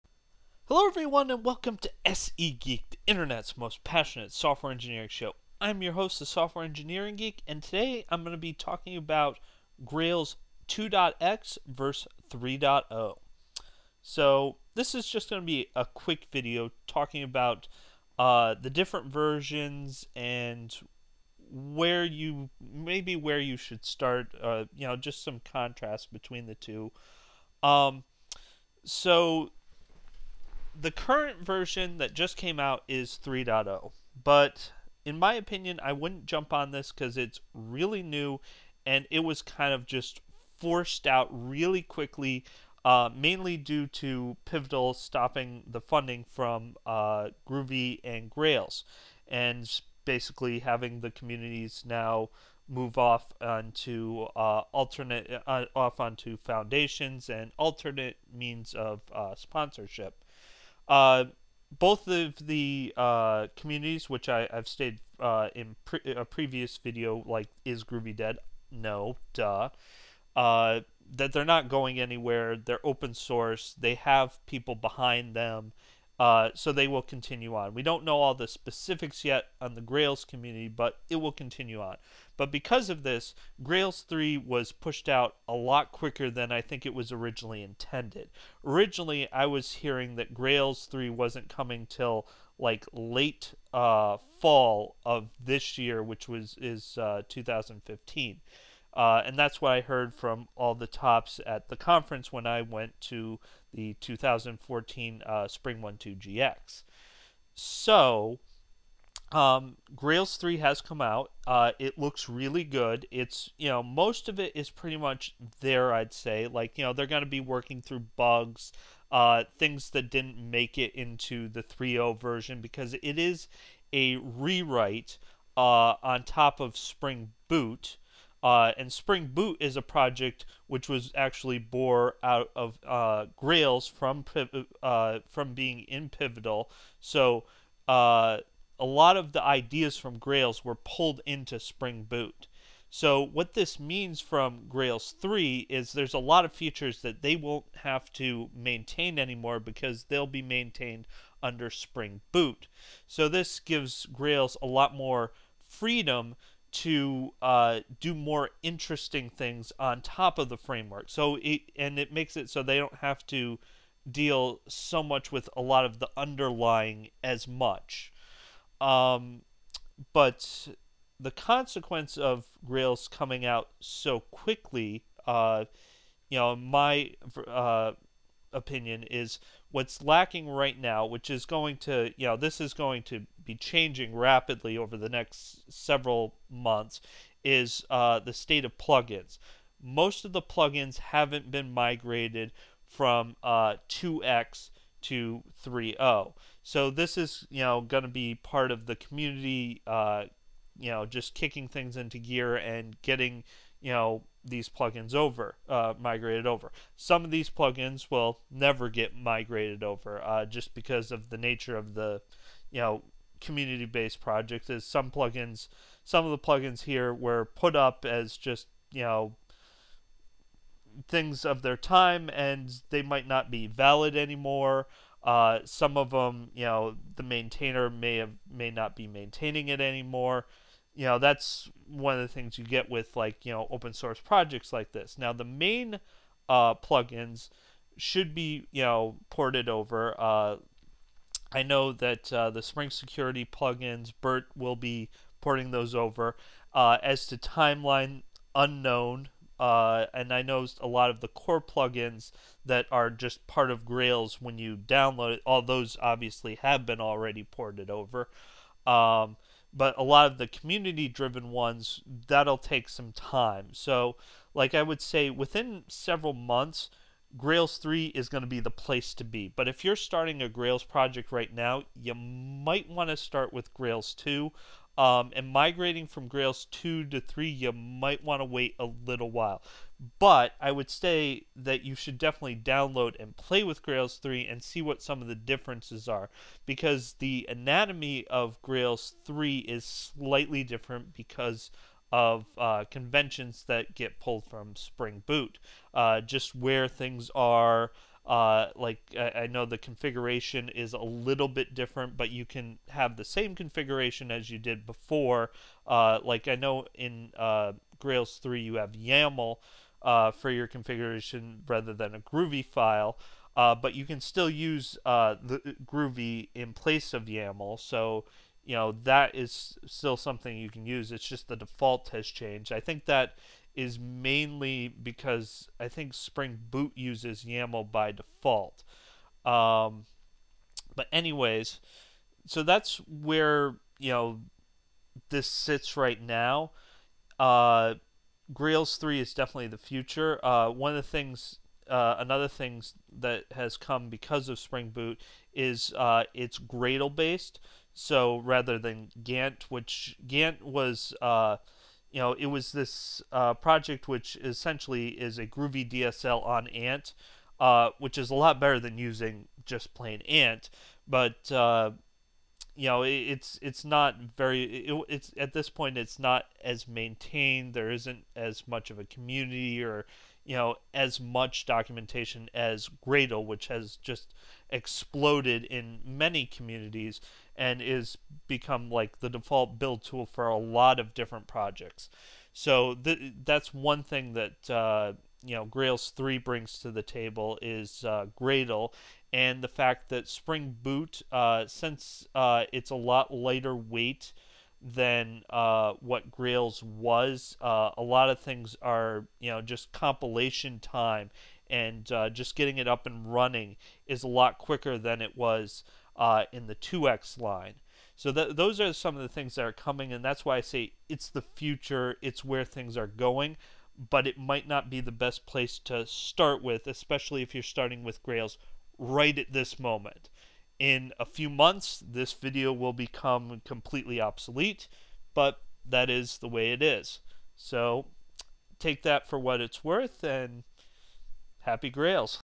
SE GEEK Episode 48 Grails 2.X vs 3.X Right Now
What is the state of Grails 2.X vs 3.X right now? Where should you start, and how will that change in the coming months?